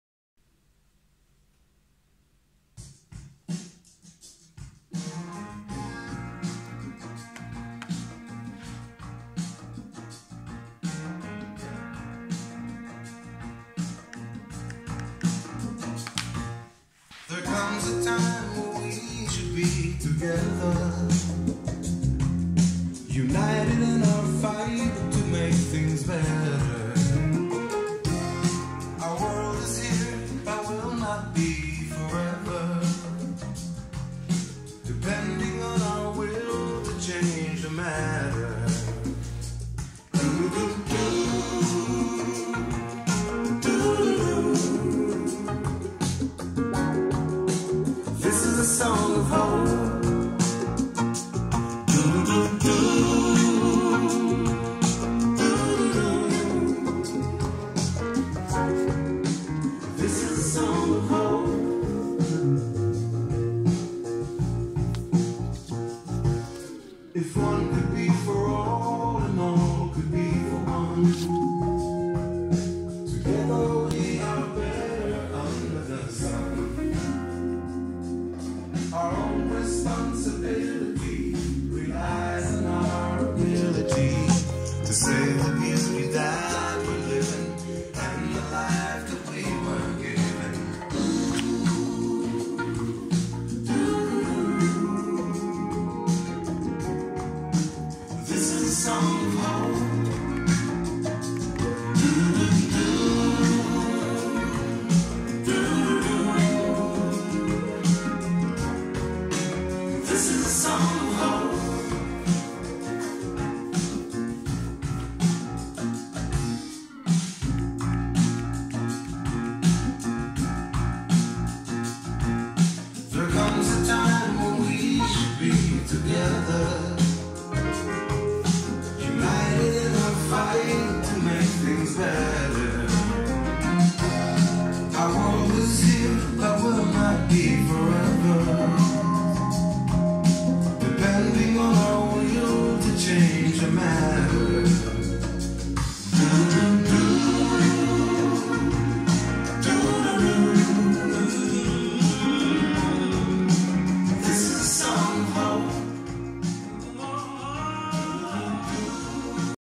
Smsl AD 18 + Focal 706 + Qobuz
Pour le fun et sachant qu'un téléphone qui filme c'est très limité, un partage sur l'association d'un SMSL AD18 avec des enceintes bibliothèques Focal.